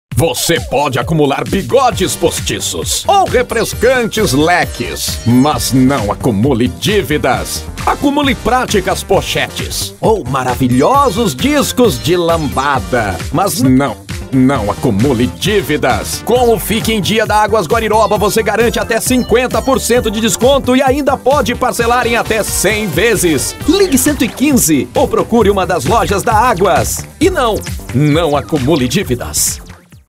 Fique em Dia 2019 - Águas Guariroba
Aproveite o Fique em Dia para quitar suas dívidas com descontos de até 50% e ainda parcelar em até 100 vezes. Tudo sem juros e multas.

Ligue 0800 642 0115 ou procure uma das lojas Águas Guariroba!